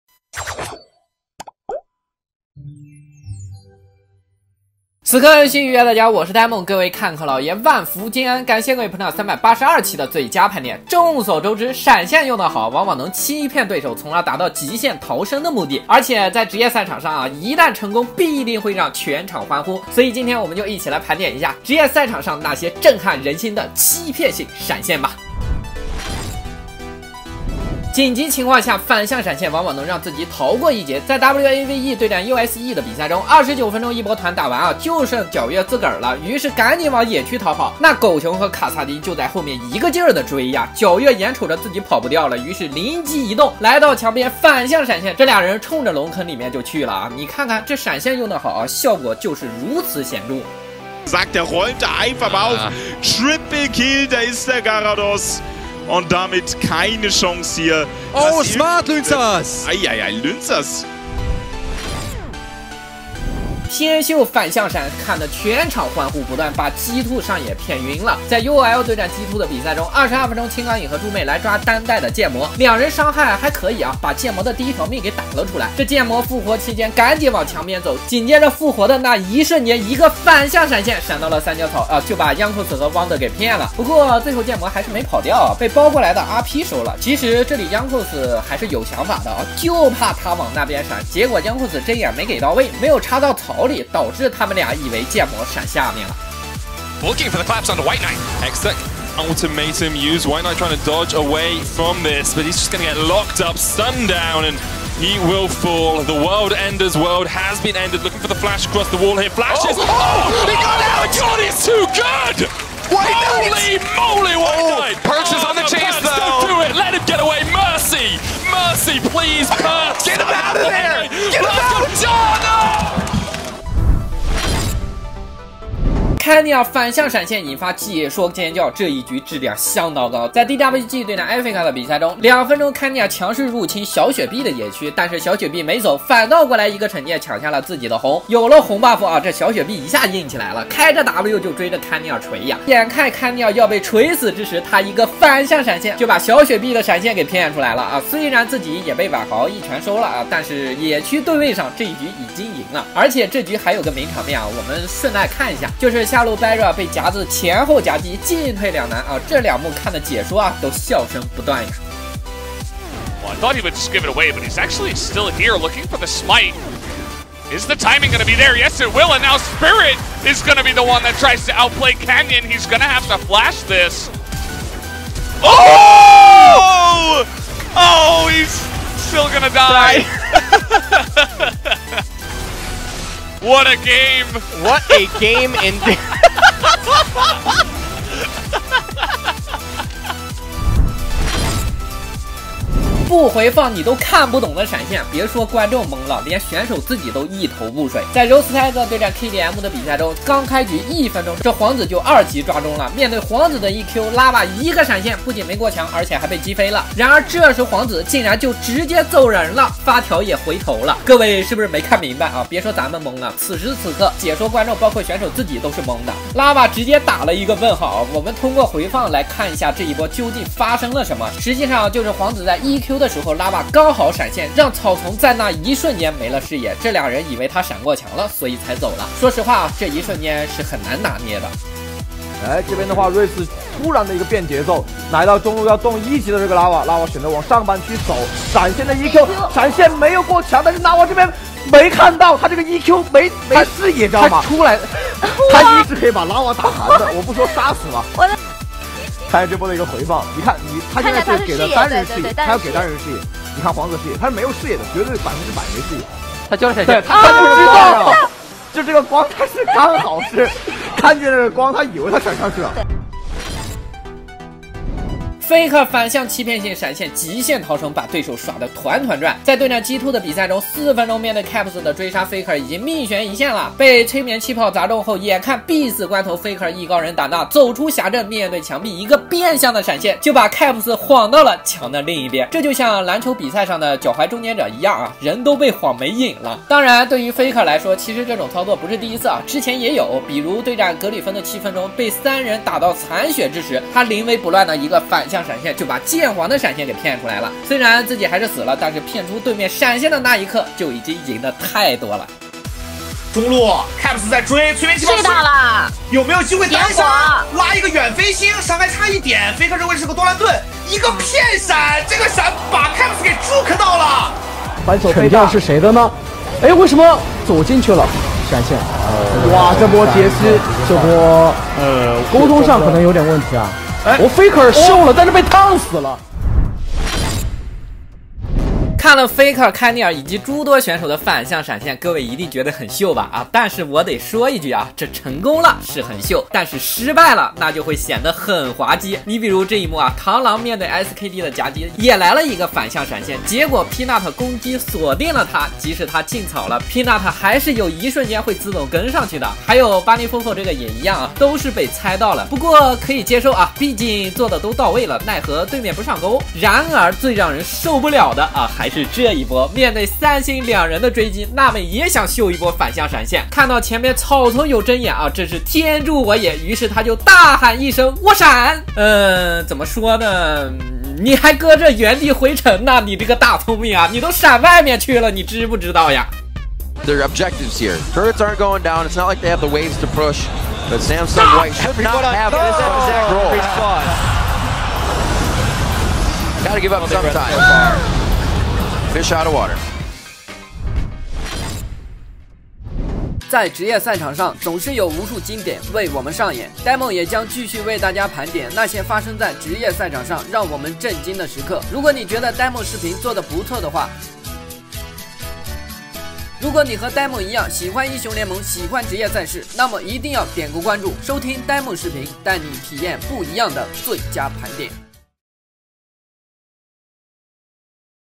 面对三人追杀，下一秒faker的反向闪现多少人看懵了？观众解说全体沸腾了
众所周知，闪现用得好，往往能欺骗对手，从而达到极限逃生的目的，而且在职业赛场上，一旦成功必定会让全场欢呼，所以今天我们就一起来盘点一下，职业赛场上那些震撼人心的欺骗性闪现吧！